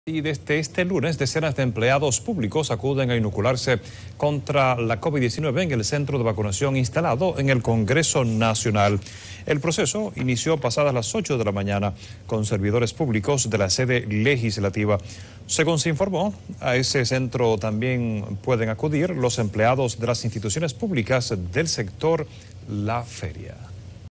Comienza a funcionar centro de vacunación instalado en el Congreso Nacional
Santo Domingo. - Desde este lunes decenas de empleados públicos  acuden  a inocularse contra la COVID-19 en el centro de vacunación  instalado en el Congreso Nacional.

El proceso inició pasada las ocho de la mañana con servidores públicos de la sede legislativa.